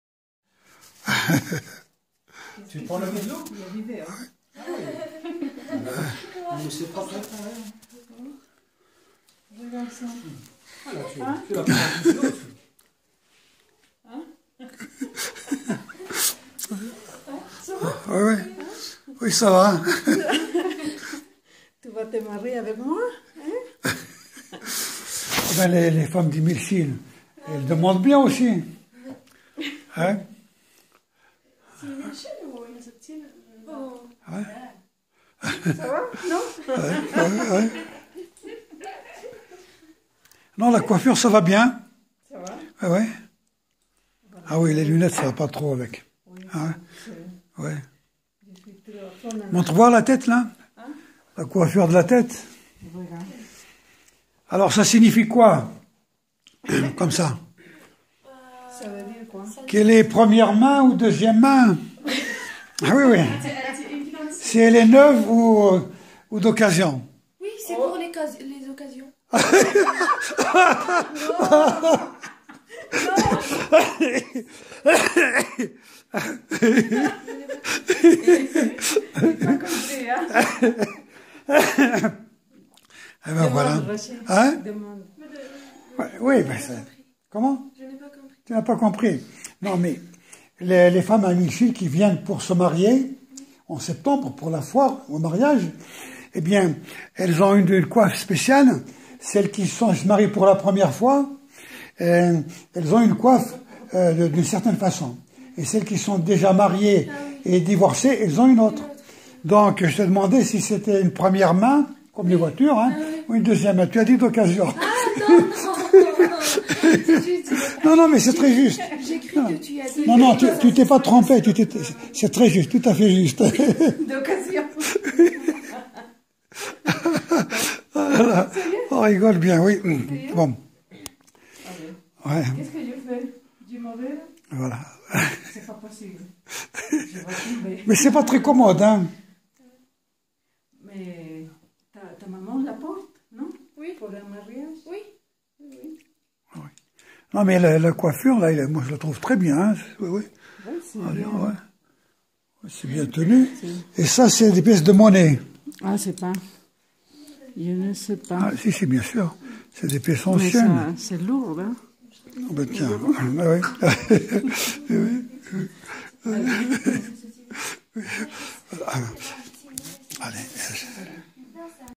2016 03 14 chez Famille Akesbi
Voir sur Facebook "Omar Akesbi" professeur. Il enseigne le français et l''arabe à des enfants berbères comme lui mais pas leur langue qu'ils ne savent pas écrire.
Pour compenser la frustration il peint, il dessine, il chante fait des recherches sur l'histoire et la langue.berbère qui a perdu beaucoup.
Certains berbères d'Afrique du nord - surtout les tunisiens - ont complètement perdu leur langues, les autres plus ou moins.
Omar travaille beaucoup sur ce sujet pense que tout peut être récupéré car ce qui est perdu par les uns est gardé par les autres. Il fait aussi des traductions.